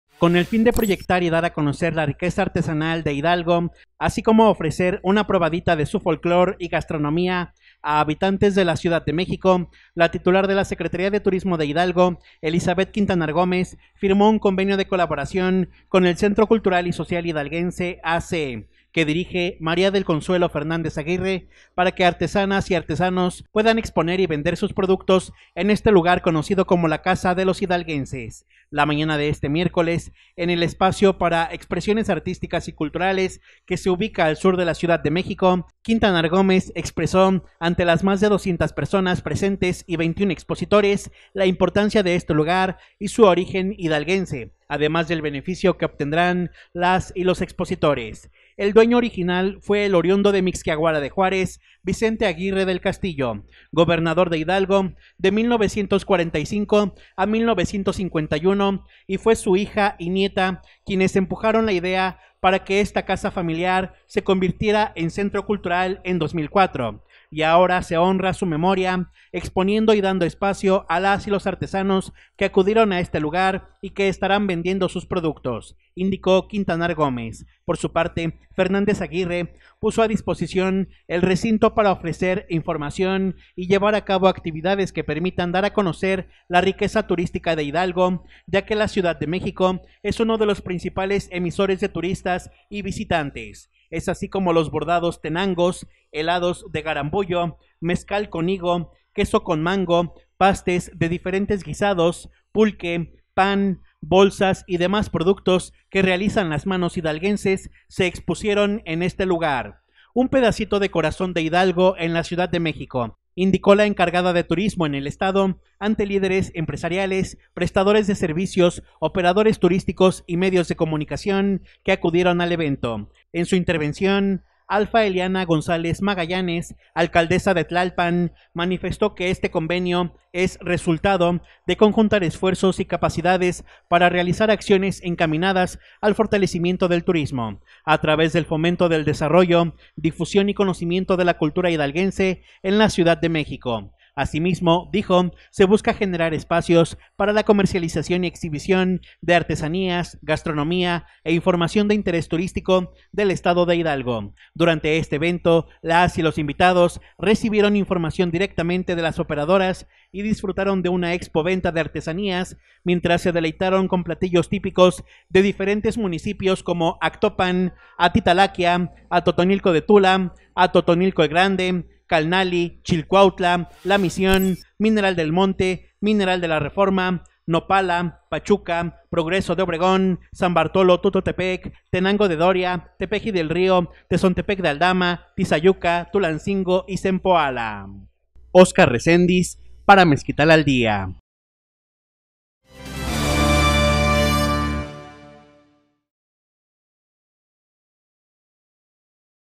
Productos artesanales de Hidalgo se exhibirán y venderán en CDMX
Con el fin de proyectar y dar a conocer la riqueza artesanal de Hidalgo, así como ofrecer una “probadita” de su folclor y gastronomía a habitantes de la Ciudad de México, la titular de la Secretaría de Turismo de Hidalgo, Elizabeth Quintanar Gómez, firmó un convenio de colaboración con el Centro Cultural y Social Hidalguense A.C., que dirige María del Consuelo Fernández Aguirre, para que artesanas y artesanos puedan exponer y vender sus productos en este lugar conocido como la Casa de los Hidalguenses.